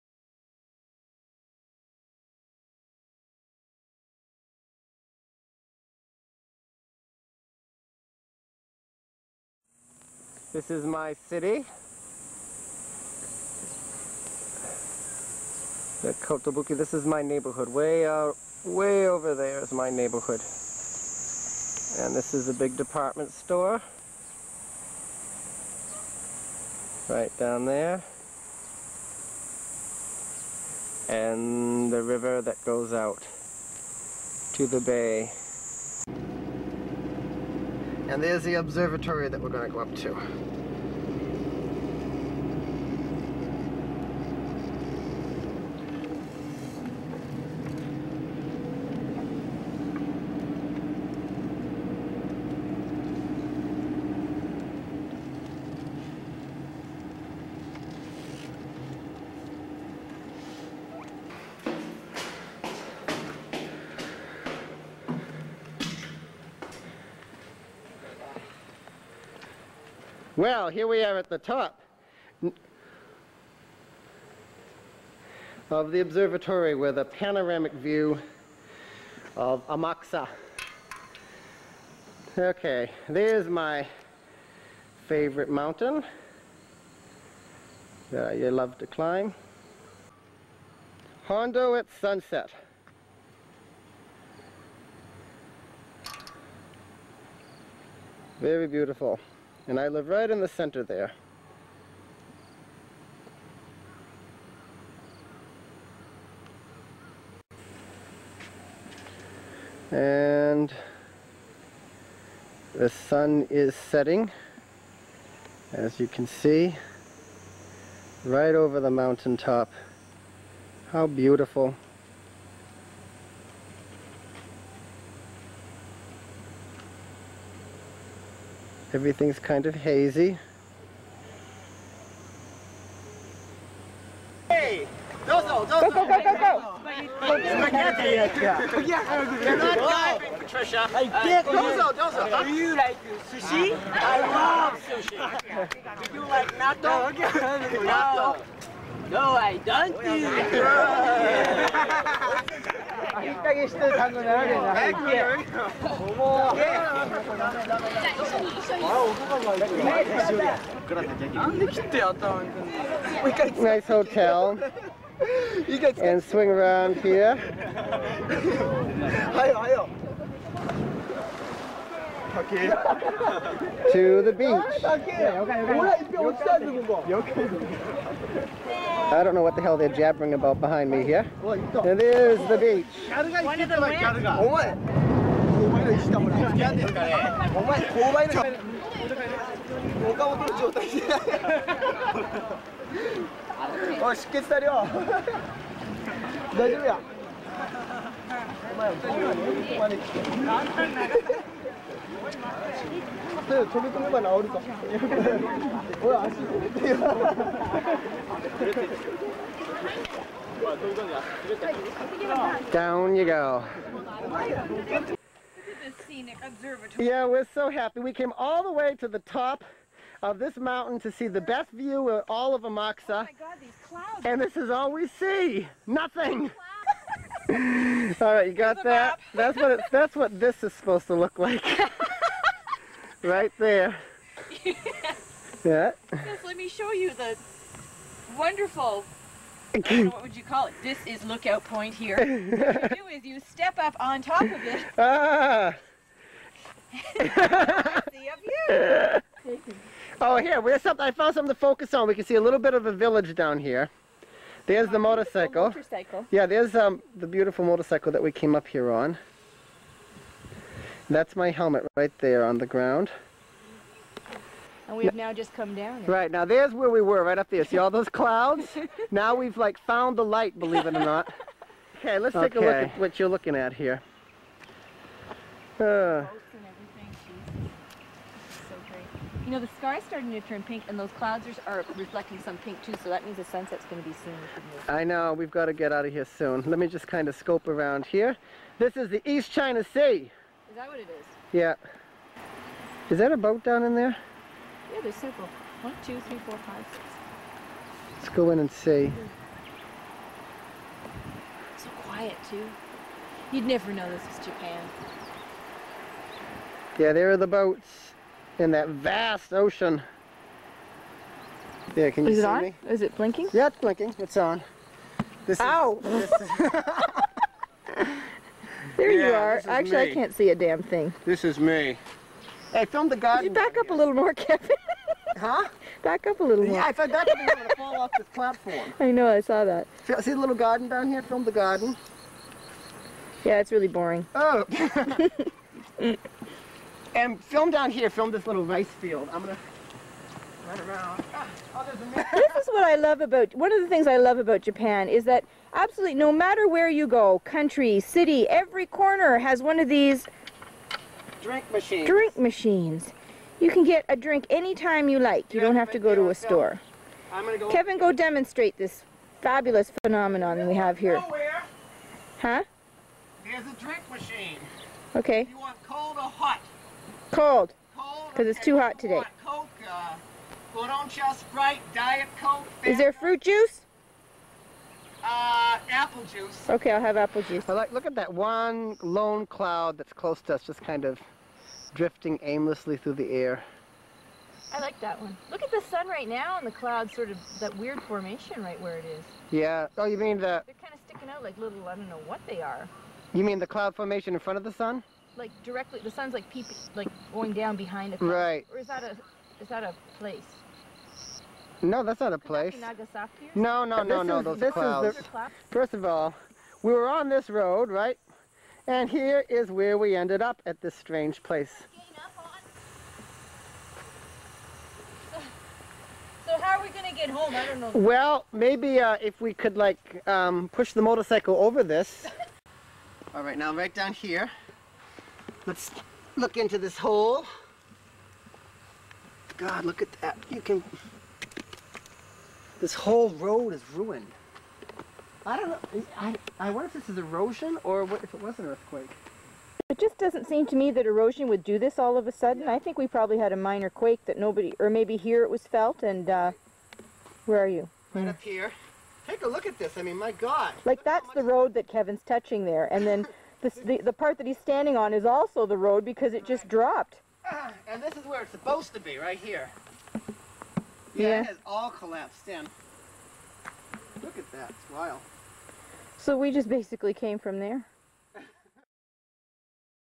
1993 -East China Sea, Amakusa, Japan - Great times
Part 1: the town I lived in from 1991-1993, Hondo, in Amakusa, a region in the prefecture of Kumamoto. I taught English there in the JET program.
Part 2: Brief snippet of the beach I used to go to a lot.
Part 3: My friend Patricia and I take a motorcycle trip to the shore of the East China Sea.